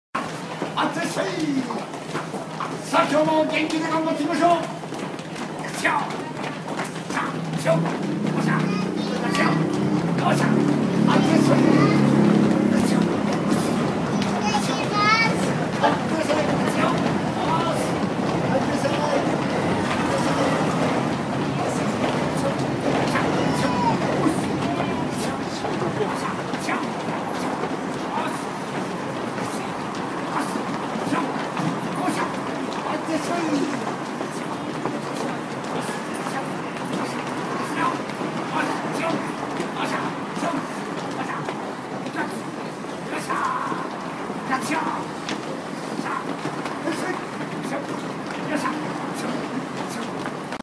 Sushi shop man yells at Tokyo commuters
Gung-ho sushi shop man outside a Tokyo railway station shouts encouragement at morning commuters.